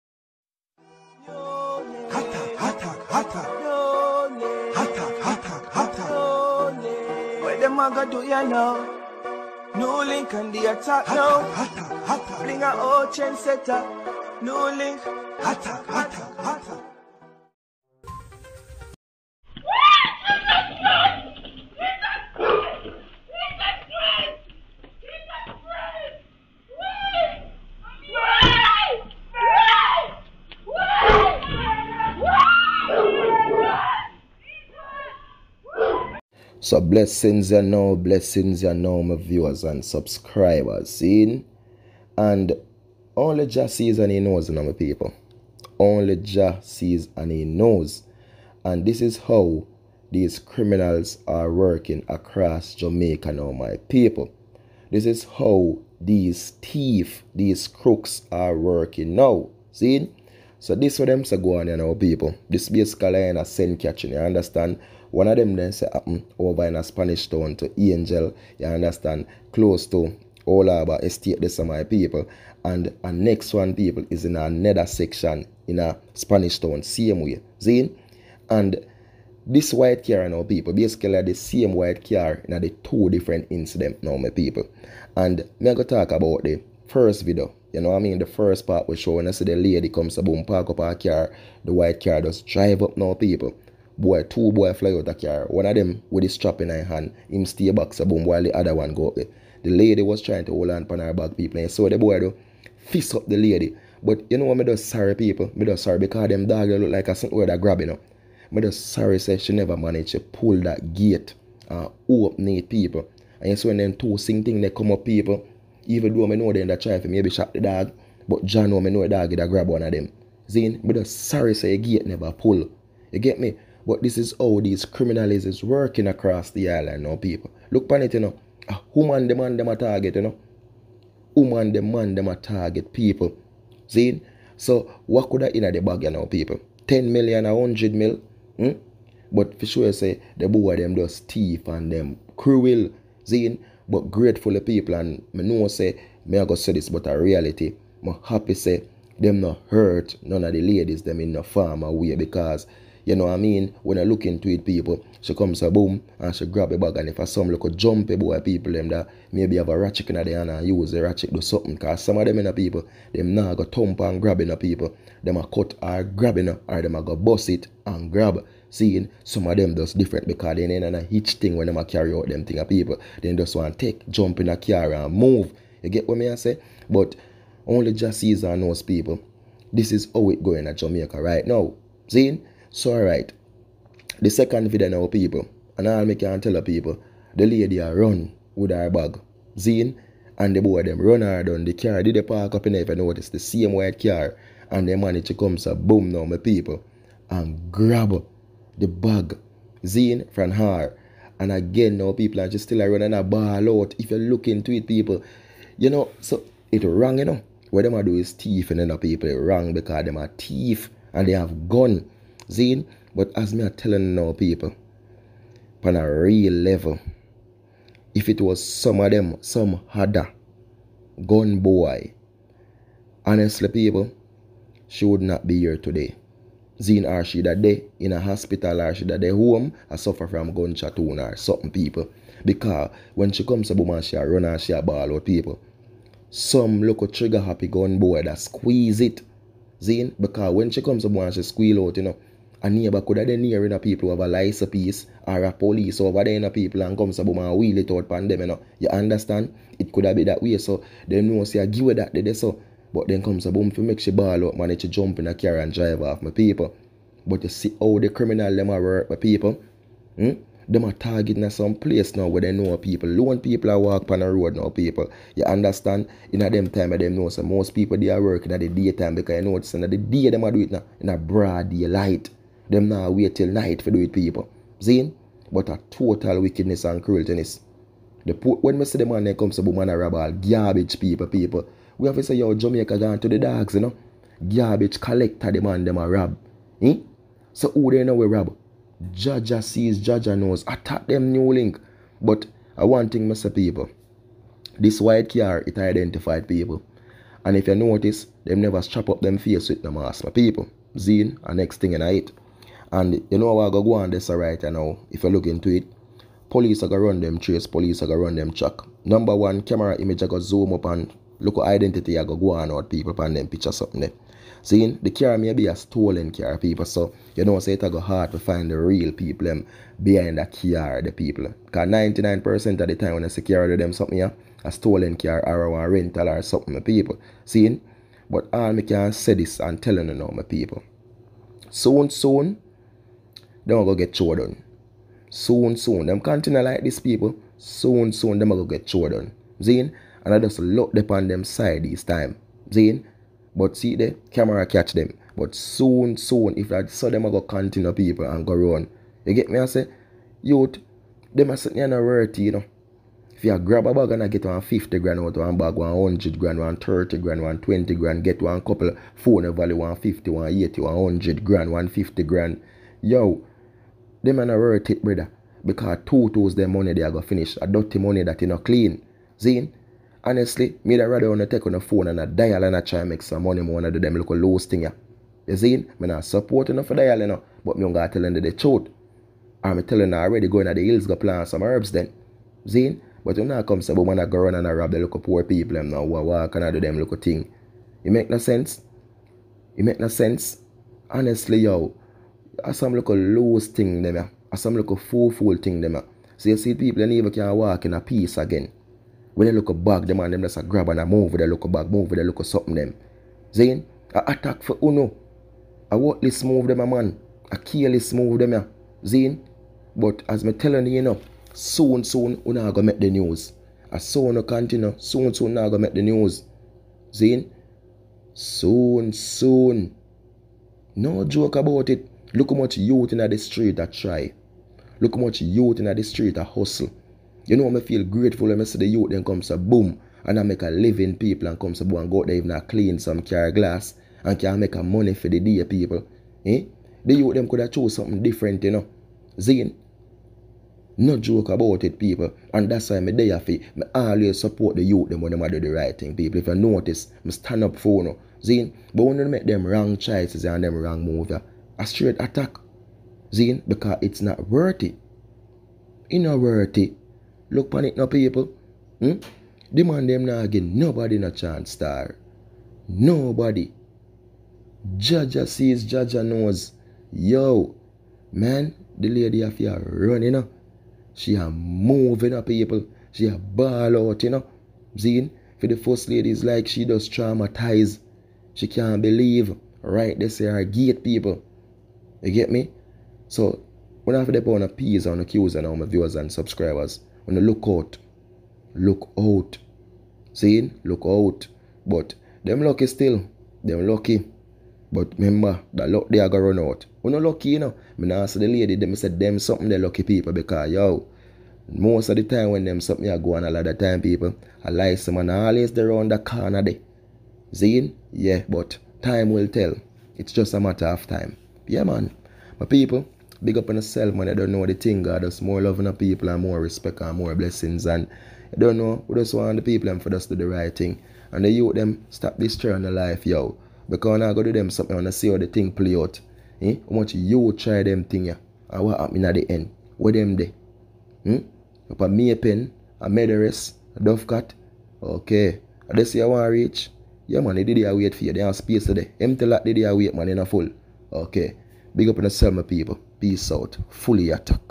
OMG W@TCH D3ADLY R0BBE3RS STRIKE IN BR0AD DAYLIGHT IN SPANISH T0WN JAMAICA
GREETINGS MY VIEWERS AND SUBSCRIBERS TODAY I SHARE WITH YOU ANOTHER STORY...THIS TIME IT'S ABOUT 2 SEPARATE INCIDENTS THAT TOOK PLACE IN ST.CATHERINE JAMAICA...PLEASE WATCH AS I SHARE MY THOUGHTS AND VIEWS TOWARDS IT.